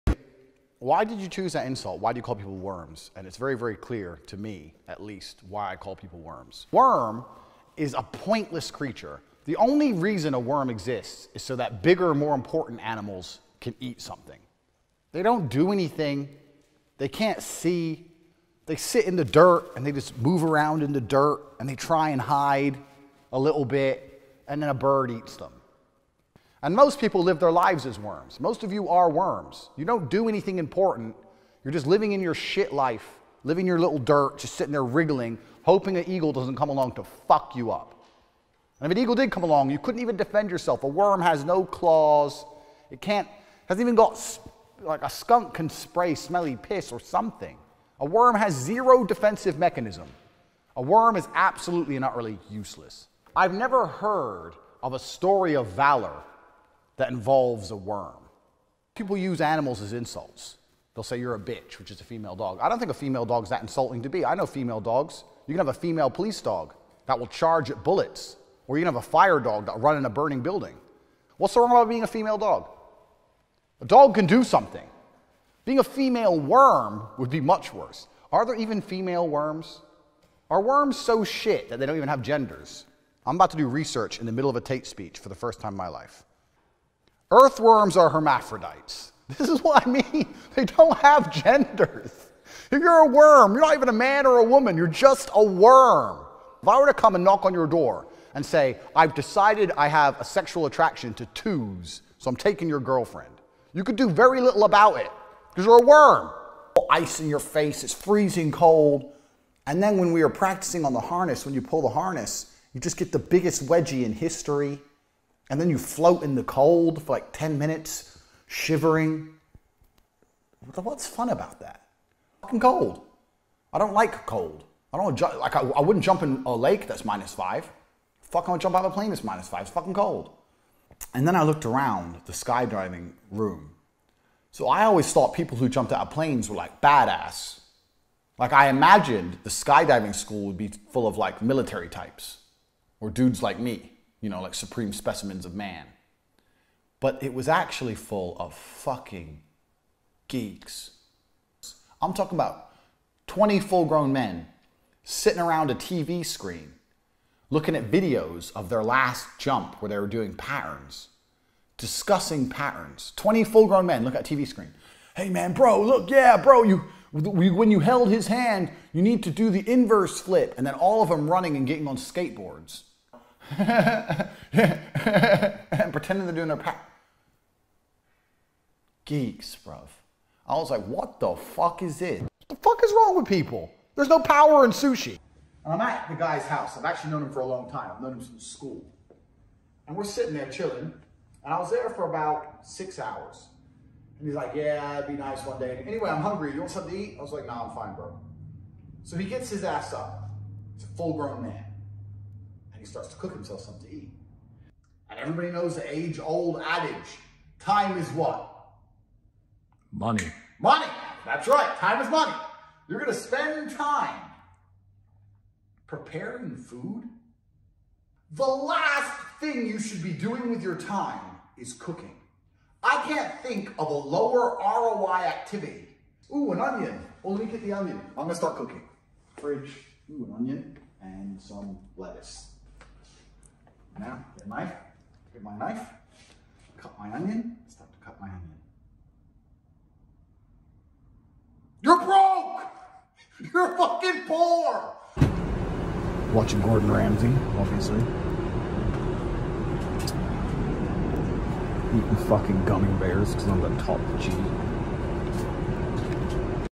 Andrew Tate | The Ultimate Compilation of Andrew Tate's Best Funny, Offensive, and Savage Moments II
Andrew Tate's Ultimate TikTok Compilation: The Best of His Funny, Offensive, and Savage Moments.

Get ready for a good laugh! This video features the best of Andrew Tate's TikTok moments from 2022. Watch as he brings his humor, offensive jokes, and savage comebacks to the forefront in this ultimate compilation. Whether you're a fan or just looking for some entertainment, this video is sure to have you in stitches. But beware this video is not for everyone